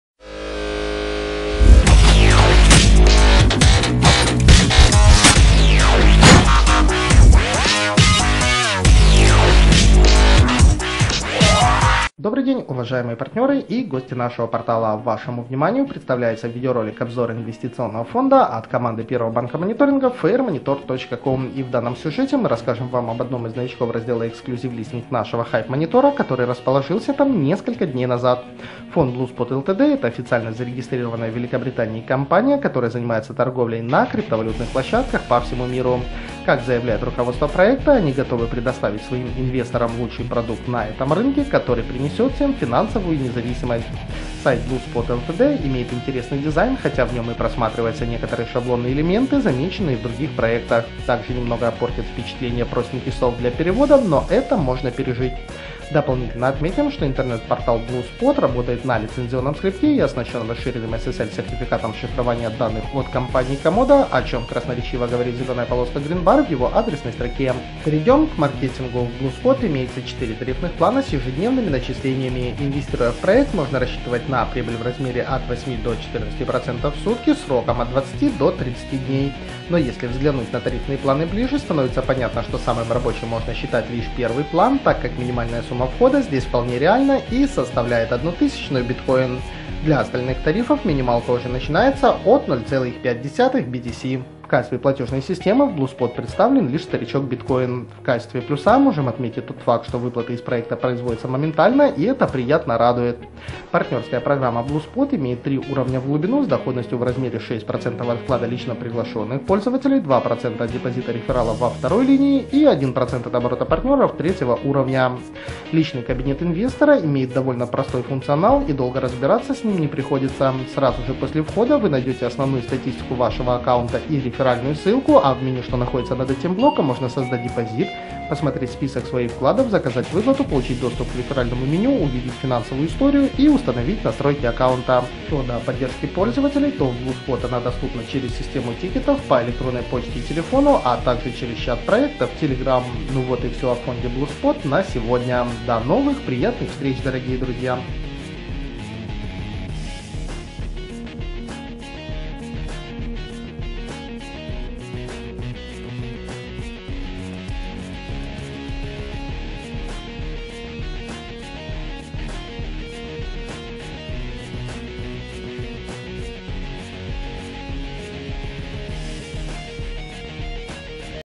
"Blue Spot Ltd" - SCAM
Проект "Blue Spot Ltd" обещает до 14% прибыли в сутки. Заманчиво? На деле, не все так просто. В этом видео мы расскажем сколько реально  можно заработать с "Blue Spot Ltd".